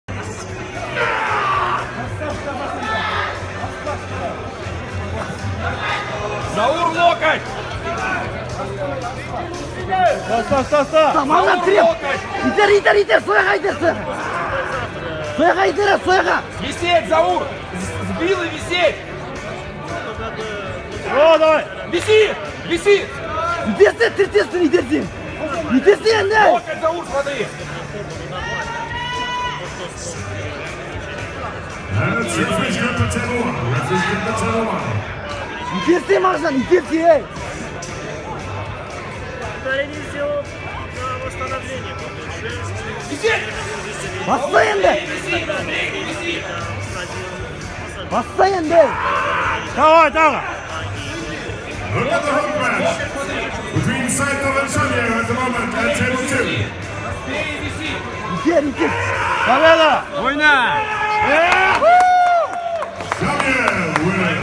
Армрестлингтен XIV әлем Кубогы
Жарыс нәтижесінде біздің құрама 4 алтын, 4 күміс, 1 қола медальдарға қол жеткізді. Жеңімпаздардың бірі - академияның 2 курс магистранты, «Шардара армспорт» клубының спортшысы Шамиев Мағжан, оң қолмен қол жығысу жарысында III орын, ал сол қолмен II орынды иеленді.